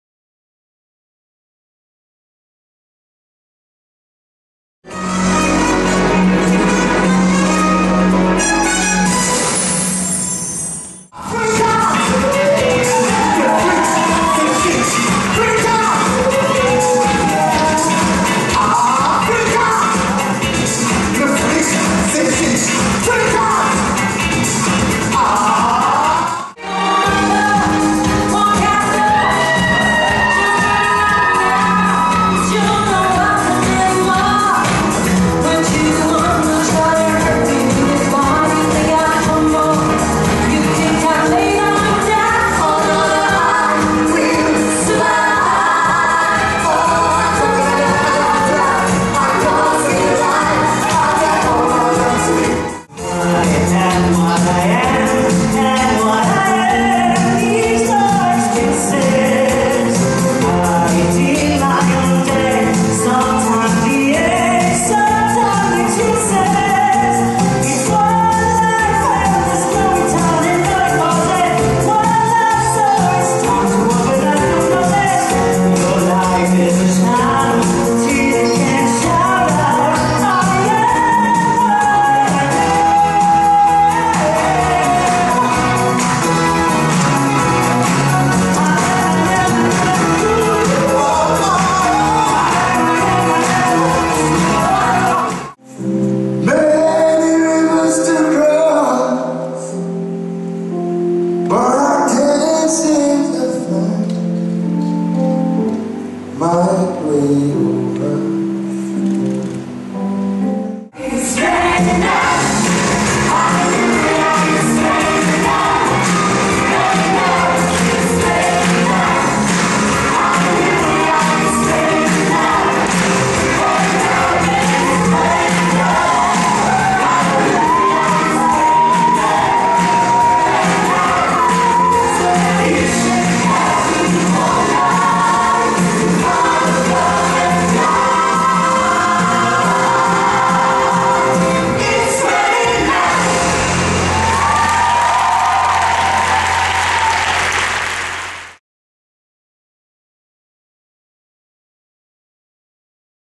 Présentation de D.I.S.C.O, le spectacle musical, aux Folies Bergère
Présentation de D.I.S.C.OO aux Folies Bergère, 19 septembre 2013.